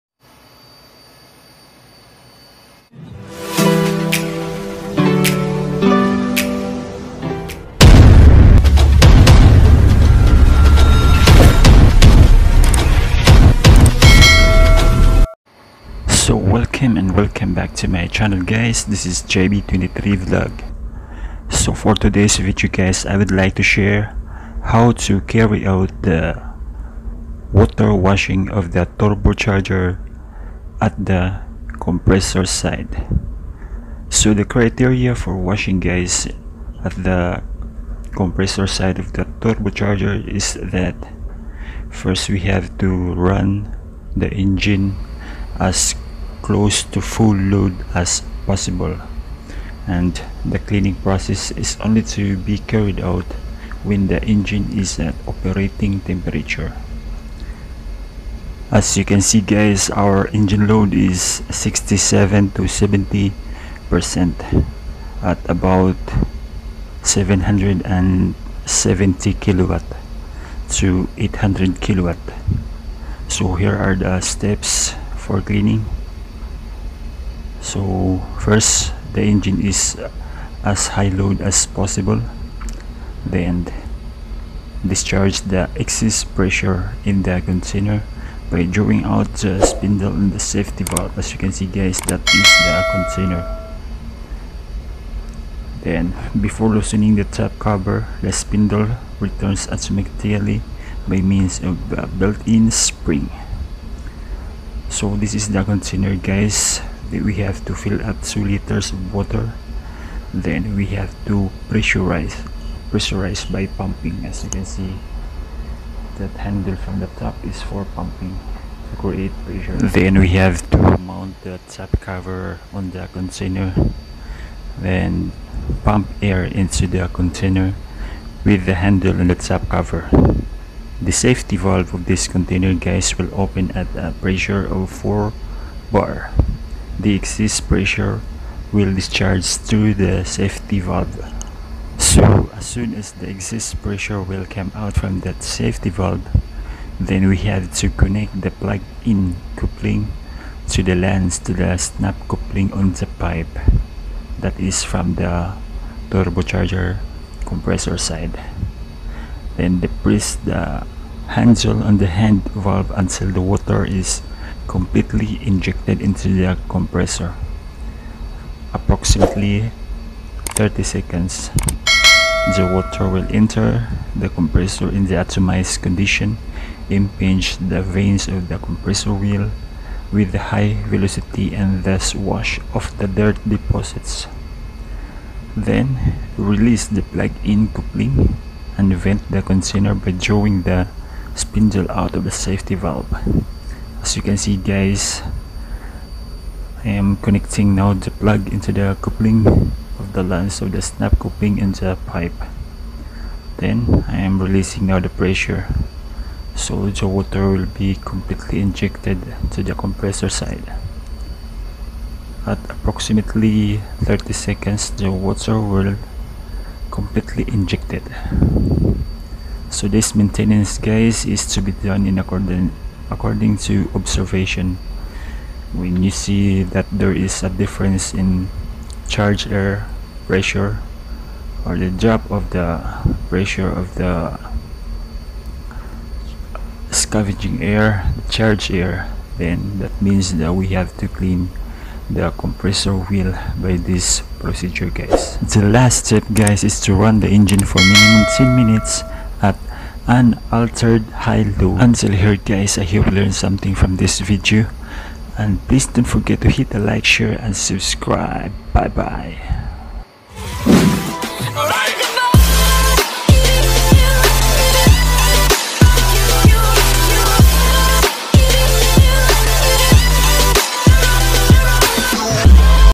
HOW TO CARRY OUT WATER WASHING OF TURBOCHARGER COMPRESSOR SIDE
you may watch overhauling videos here in this link

BOILER SWIRLER INSPECTION